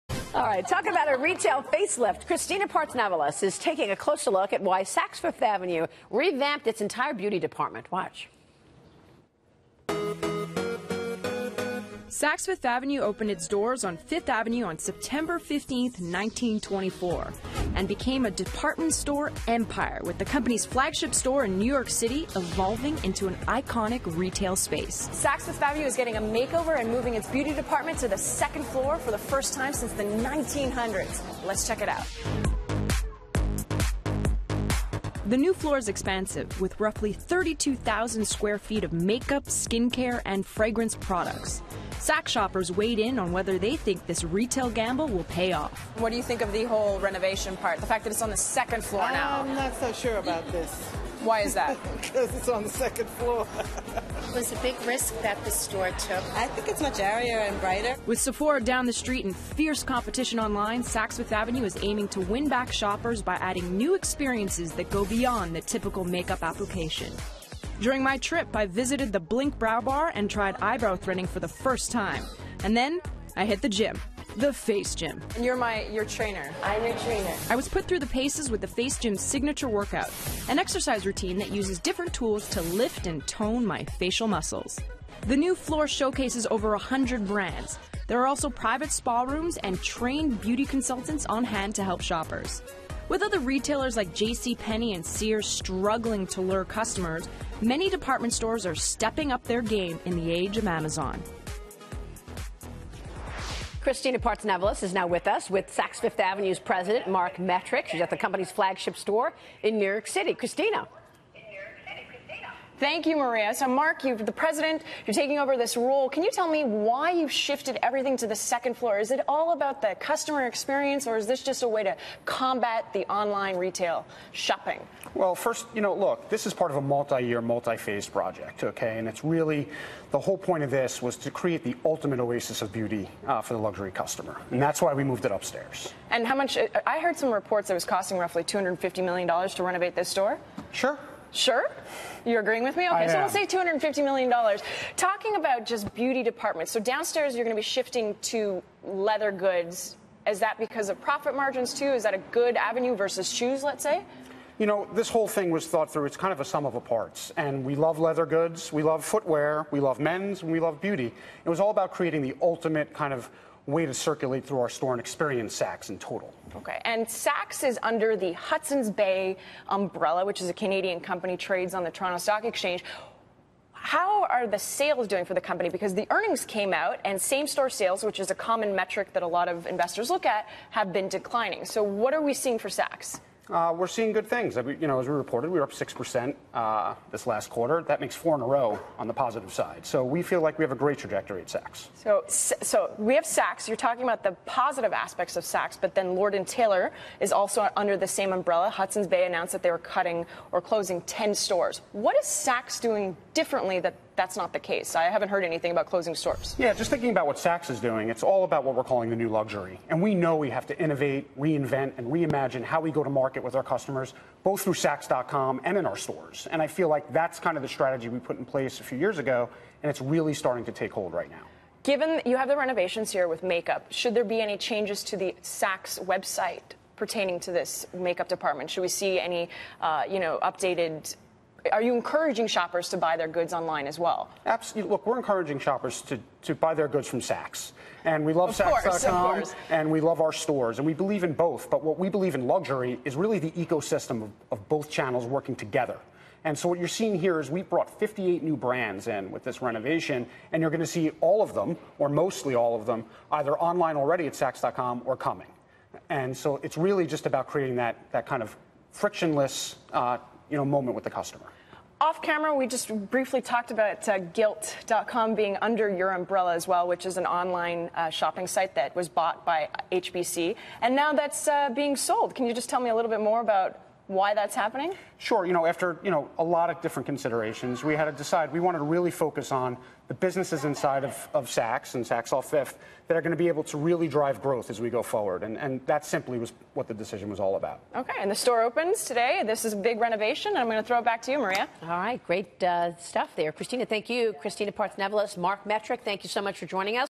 Saks Fifth Avenue beauty department gets makeover
FBN's Kristina Partsinevelos talks to Saks Fifth Avenue President Marc Metrick on the Saks beauty department makeover, the retailer's growth outlook, the success of its strategy, online sales and the company's decision to sell Gilt.com.